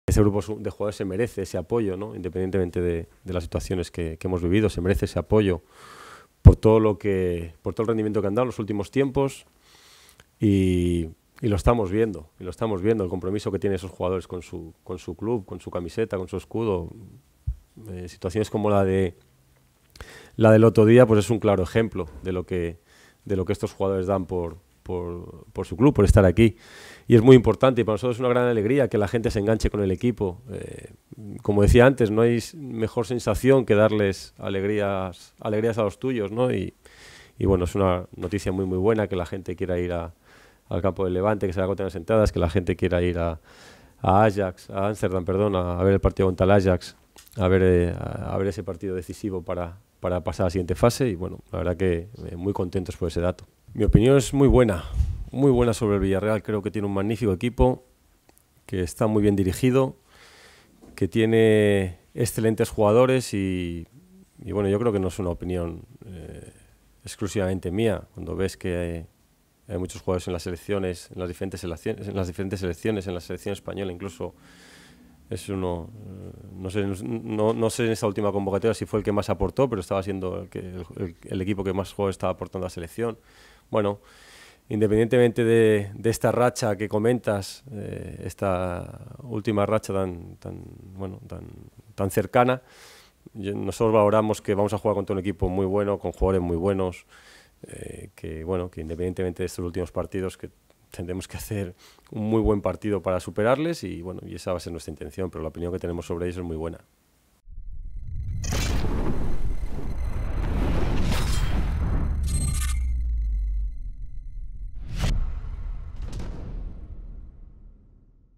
CELADES: "I HOPE WE CAN BE IN THE SAME CONDITION AS ON WEDNESDAY"
The VCF coach is looking for another strong performance from his side in the regional derby against Villarreal CF.